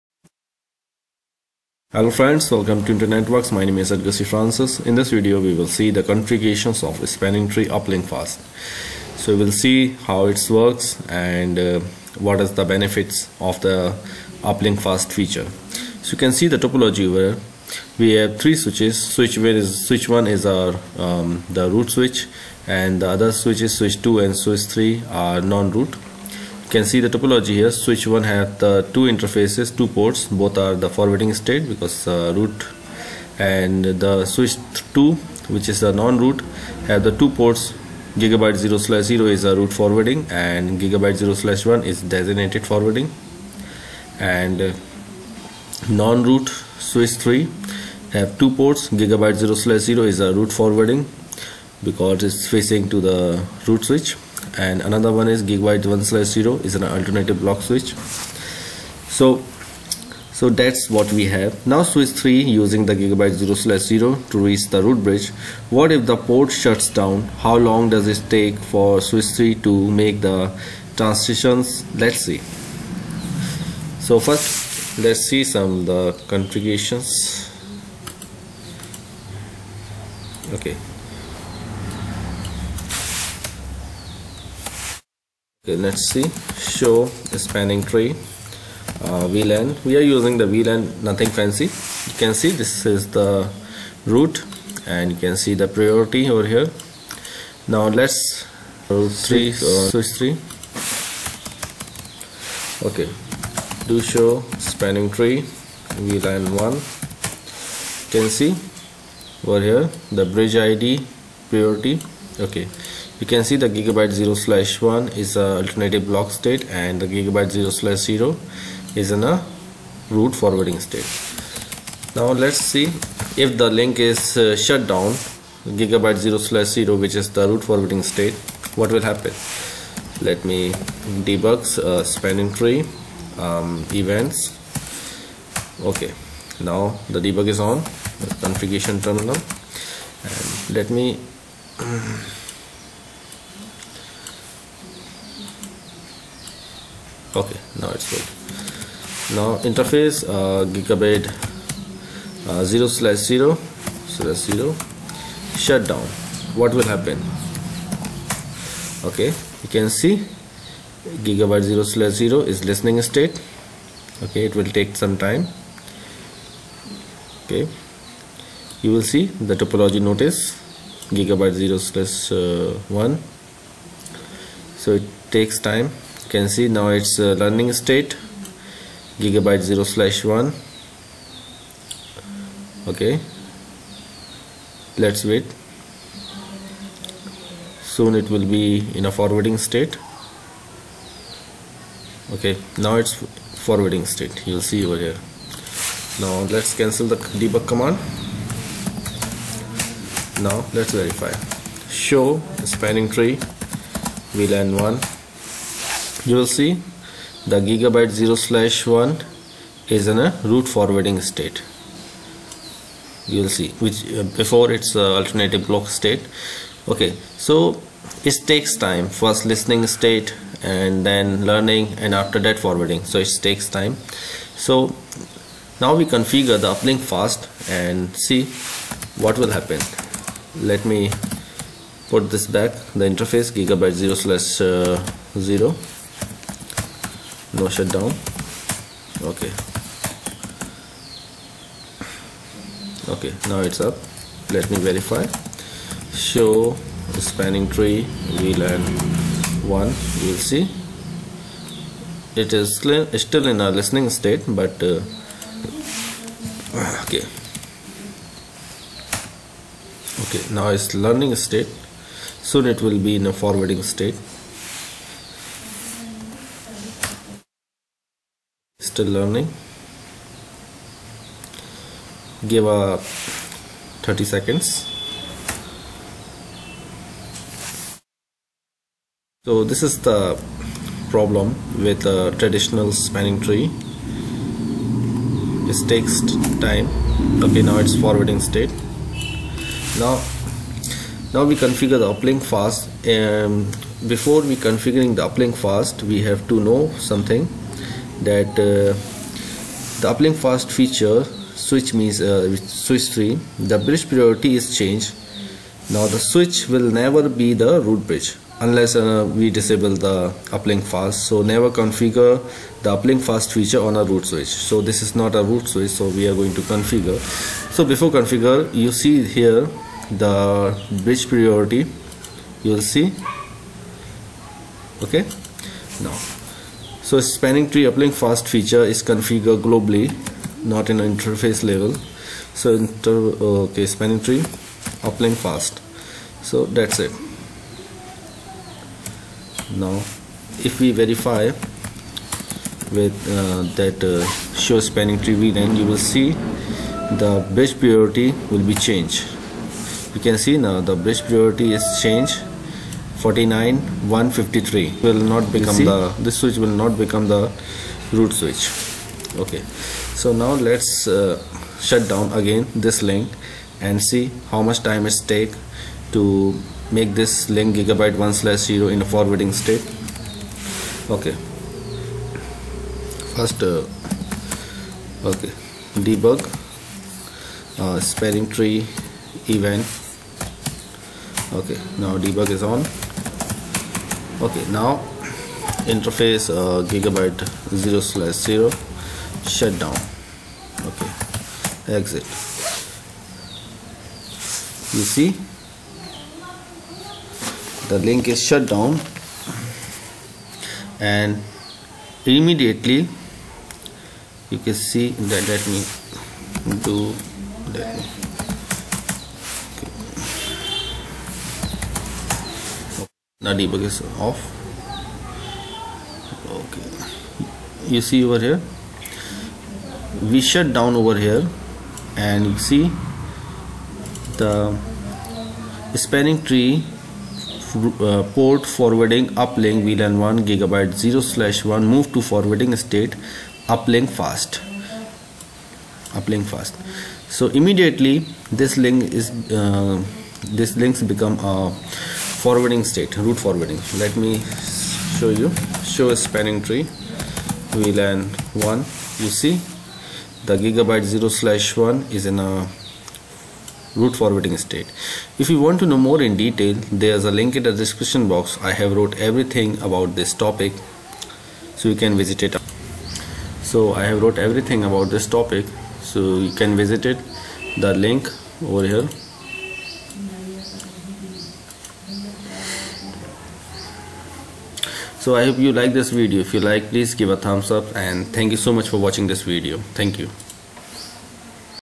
What is STP Uplinkfast? How to configure Uplinkfast?
What is uplinkfast?

Spanning tree UplinkFast is a Cisco-specific enhancement to the Spanning Tree Protocol (STP). It's designed to improve the convergence time in the event of a direct link failure, especially in access-layer switches with redundant links to the distribution layer.

Here's how it works:
Quick Recovery: When the primary uplink fails, UplinkFast allows the switch to quickly transition the blocked port to forwarding, bypassing the usual listening and learning states.

Preventing Loops: It modifies the Bridge Protocol Data Units (BPDUs) to prevent loops by increasing the path cost and setting the local bridge priority.

Immediate Restoration: This feature ensures that connectivity is restored almost immediately, reducing downtime significantly.

UplinkFast is typically used in access-layer switches and should be deployed carefully to avoid potential issues with MAC address tables.

Free Tutorials Theories and Labs

1. Complete CCNA CCNP and CCIE

2. CCIE LABs

3. MPLS full course

4. DMVPN

5. QoS

6. IPSec